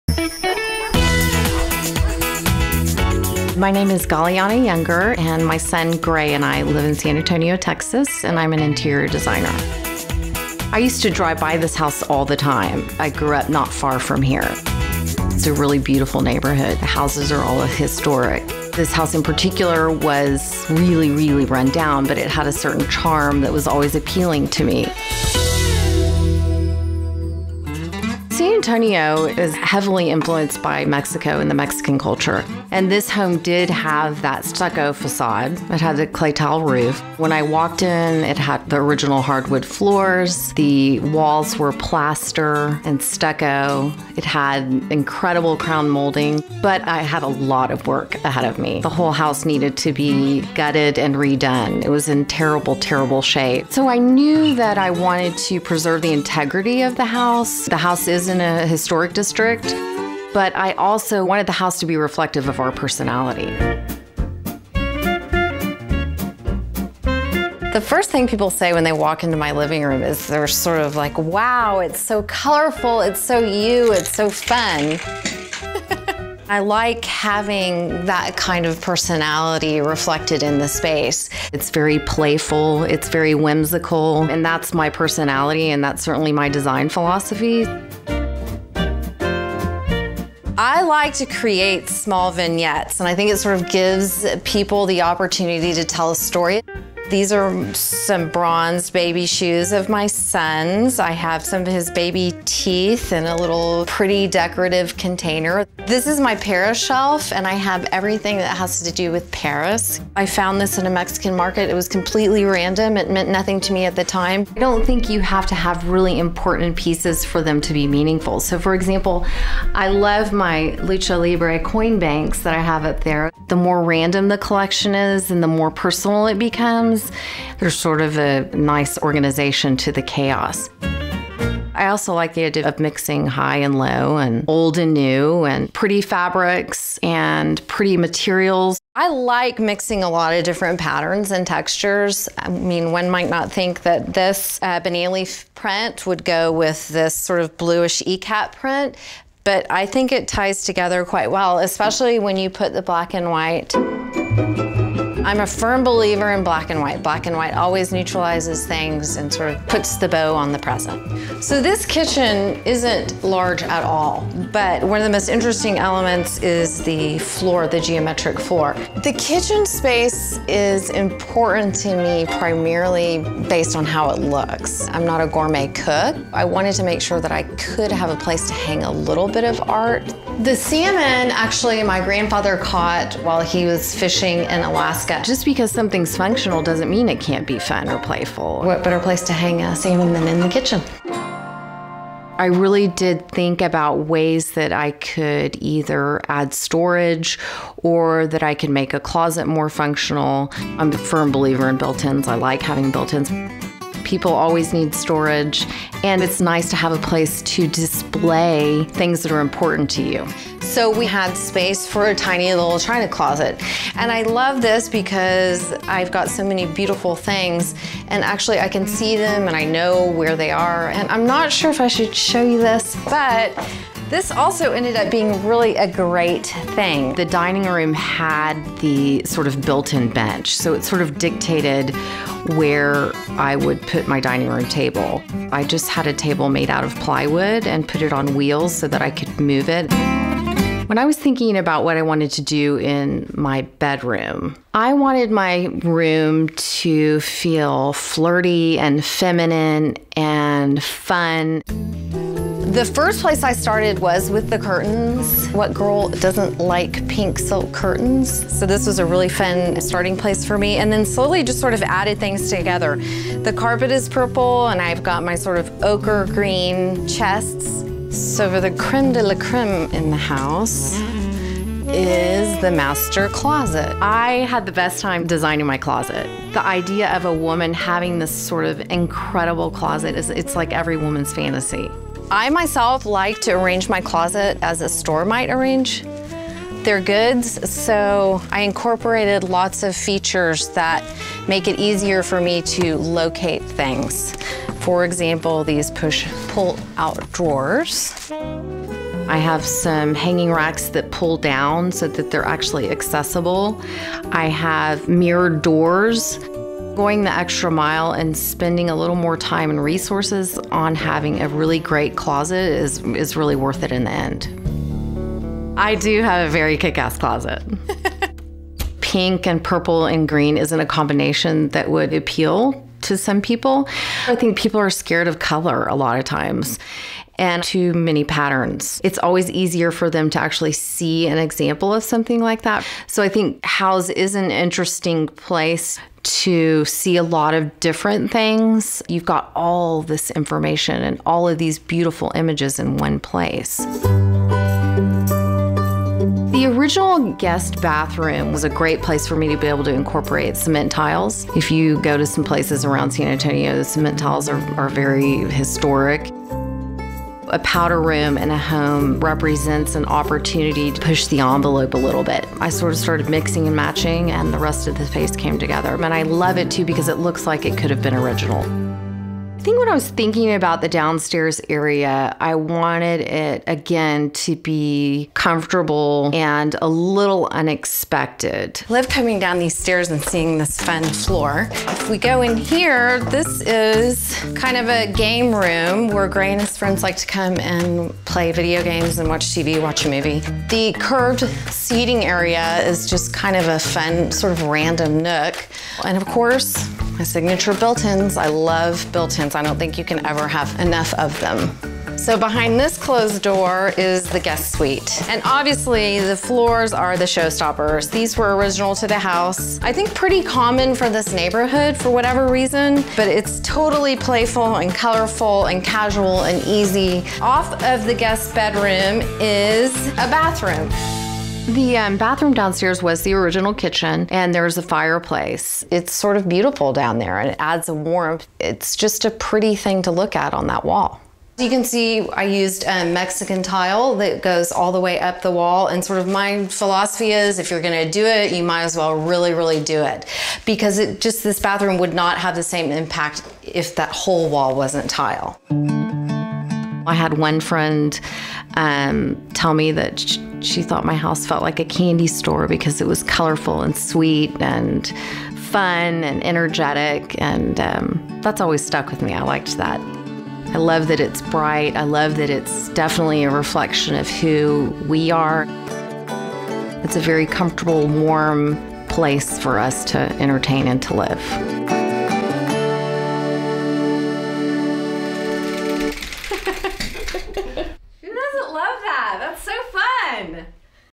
A Spanish-Style Home Full of Color and Joy in San Antonio
Galeana Younger knew her 1920s home in downtown San Antonio, Texas, would have to be taken down to the studs, but she saw the potential underneath years of neglect. She gave the dated home a yearlong renovation with what she calls “unconditional love,” and the home is now full of color and whimsical touches that help many of the original features shine.

One of the things Younger likes about her home is how well it functions for her and her son. “I really think about the space and how I want it to function, and then I set out to find things,” she says. “I typically pool together lots of options before paring things down. Then I make sure my final selects tell a complete story.”